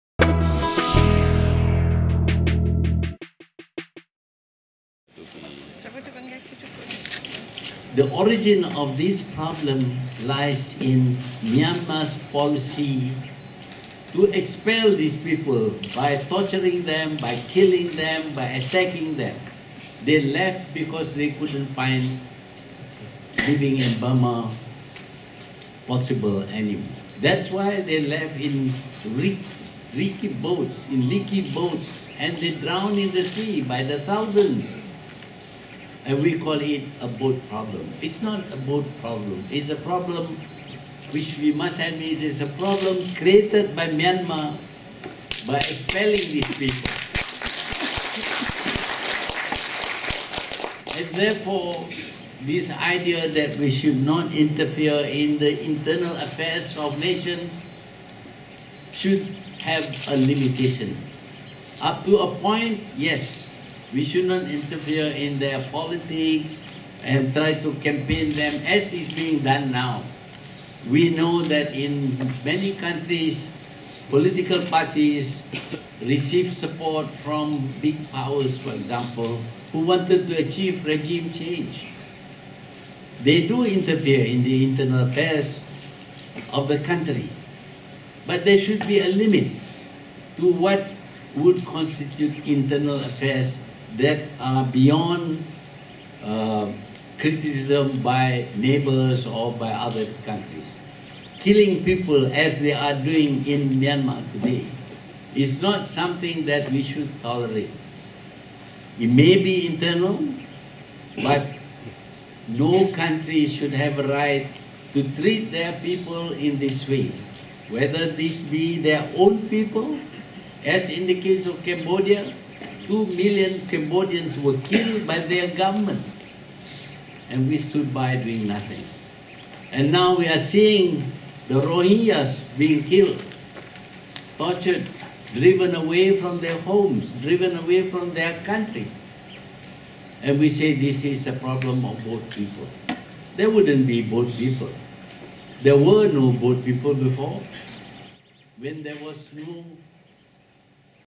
It's not an issue of "boat people" or floating coffins
Tun Mahathir Mohamad says, the United Nations and Asean in particular must draw a limit to what constitutes non-interference policy.